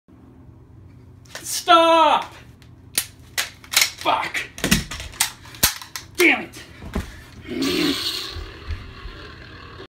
Always carry a backup/responsible concealed carry...
As a responsible armed citizen its is of the upmost importance to carry a backup to your primary firearm. Anything can happen.